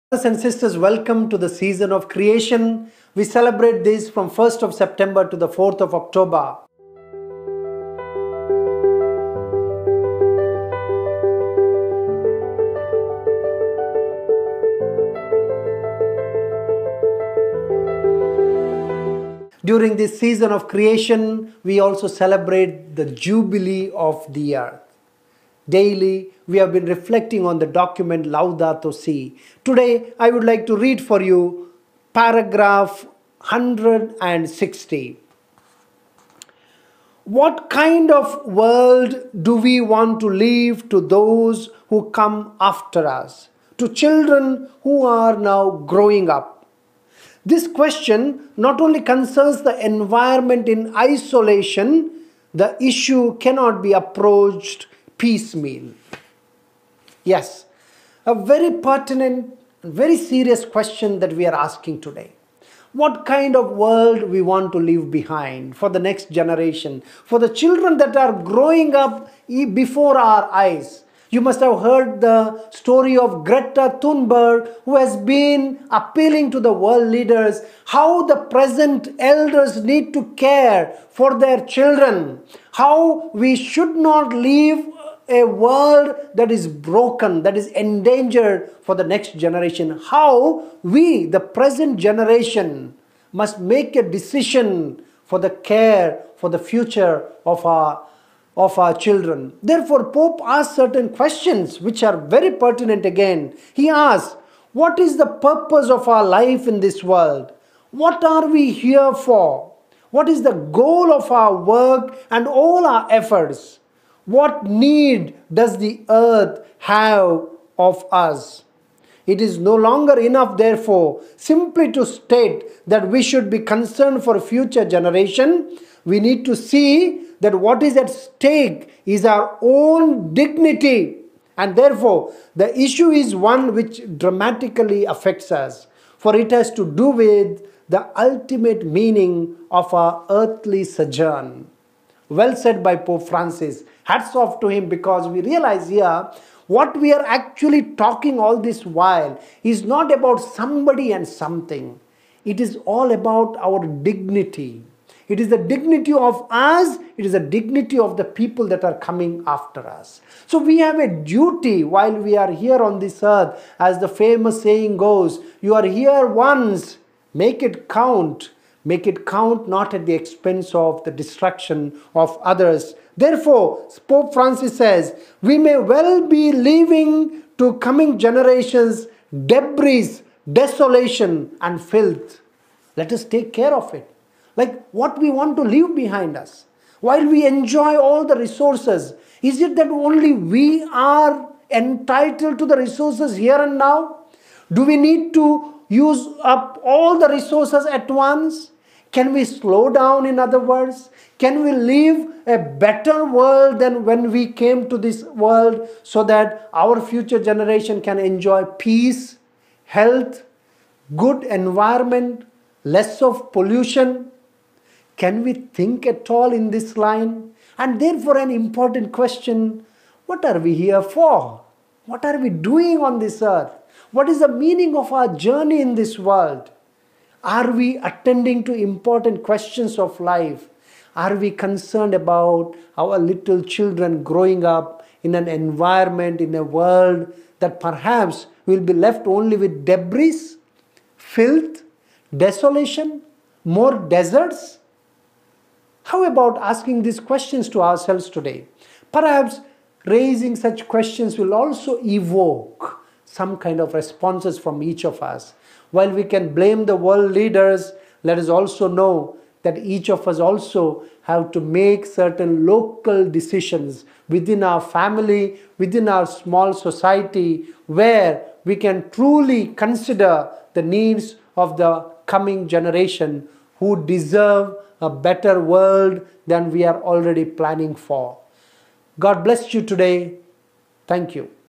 SeasonofCreation2020 | 1 Sep- 4 Oct | LaudatoSi |Fr. Praveen D Souza, OFM |FrancisofAssisi|17Sep
The Season of Creation is a time to renew our relationship with our Creator and all creation through celebration, conversion, and commitment together. During the Season of Creation, we join our sisters and brothers in the ecumenical family in prayer and action for our common home.

Ecumenical Patriarch Dimitrios I proclaimed 1 September as a day of prayer for creation for the Orthodox in 1989. In fact, the Orthodox church year starts on that day with a commemoration of how God created the world. The World Council of Churches was instrumental in making a special time a season, extending the celebration from 1 September until 4 October. Pope Francis made the Roman Catholic Church’s warm welcome of the season official in 2015.

In recent years, statements from religious leaders around the world have also encouraged the faithful to take time to care for creation during the month-long celebration. The season starts 1 September, the Day of Prayer for Creation, and ends 4 October, the Feast of St. Francis of Assisi, the patron saint of ecology beloved by many Christian denominations
Courtesy: